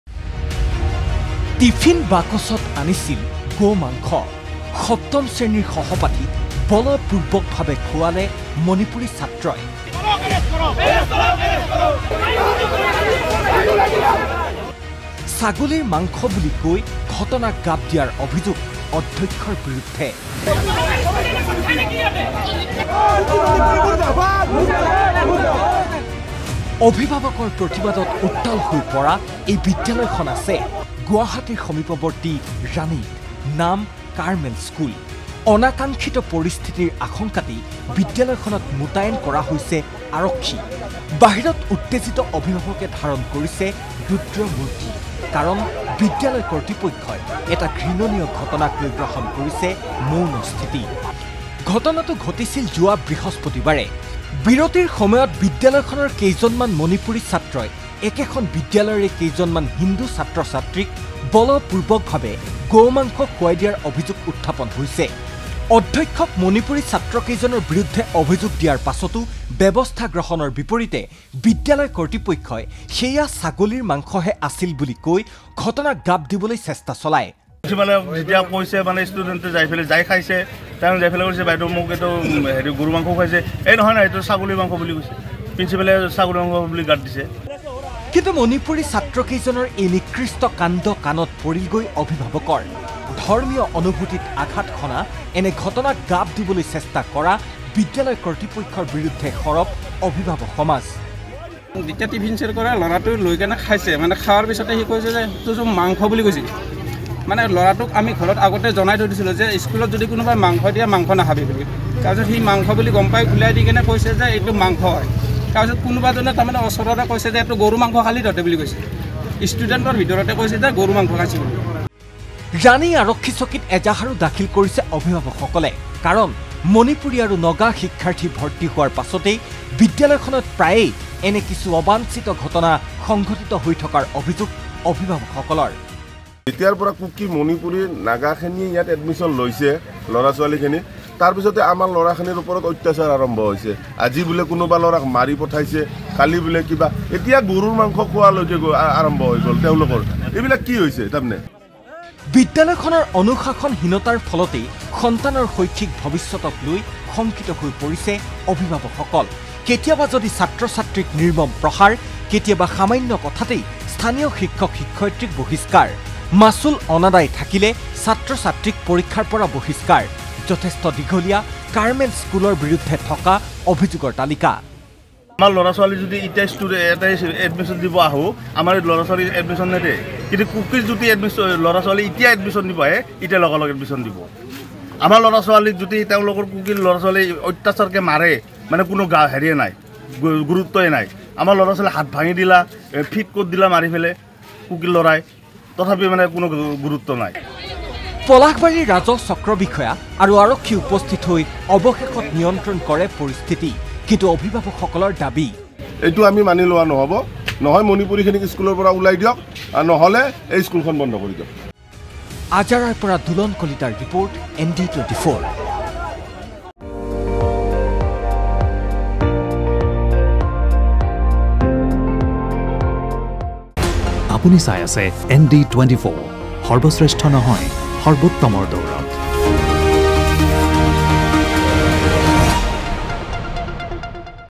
Assam News: সপ্তম শ্ৰেণীৰ সহপাঠীক বলপূৰ্বকভাৱে গো-মাংস খুৱালে মণিপুৰী ছাত্ৰই
সপ্তম শ্ৰেণীৰ সহপাঠীক বলপূৰ্বকভাৱে গো-মাংস খুৱালে মণিপুৰী ছাত্ৰই। টিফিন বাকচত আনিছিল গো-মাংস। ছাগলীৰ মাংস বুলি কৈ ঘটনাক গাপ দিয়াৰ অভিযোগ অধ্যক্ষৰ বিৰুদ্ধে। অভিভাৱকৰ প্ৰতিবাদত উত্তাল ৰাণীস্থিত কাৰ্মেল স্কুল।